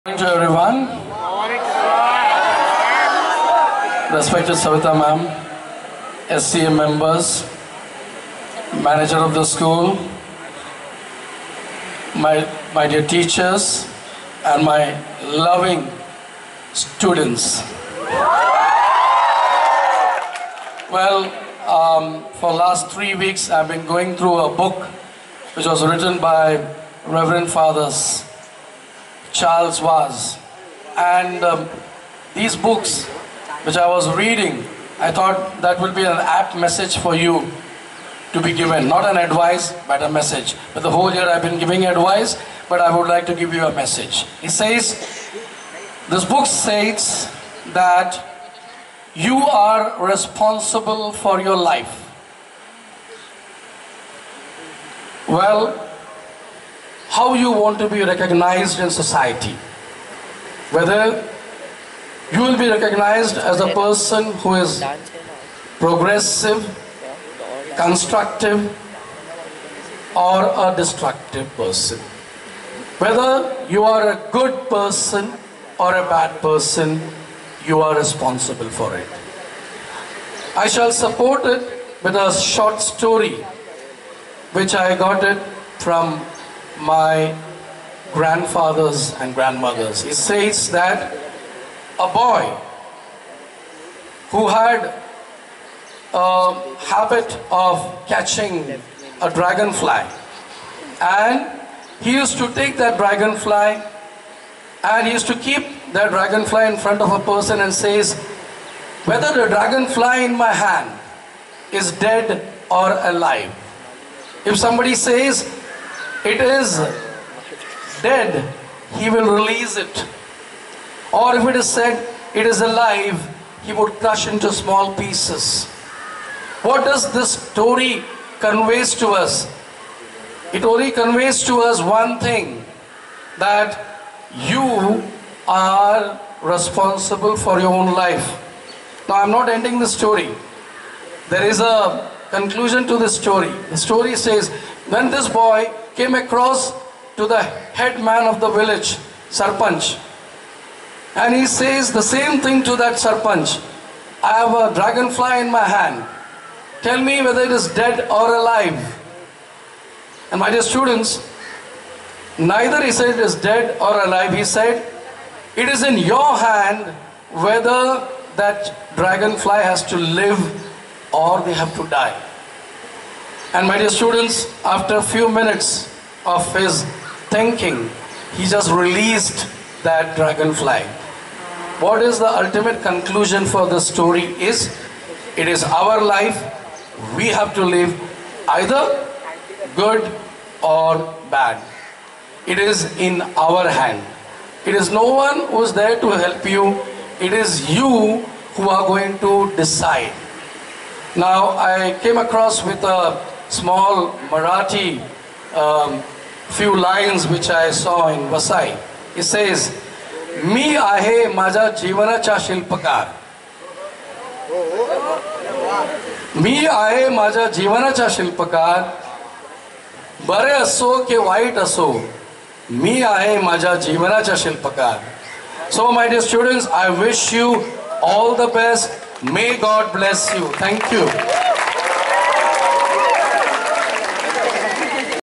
Motivational Speech by Thomas Sir in HD
This speech was given to the students on Farewell.
Thomas Sir teaches History to std-10 students.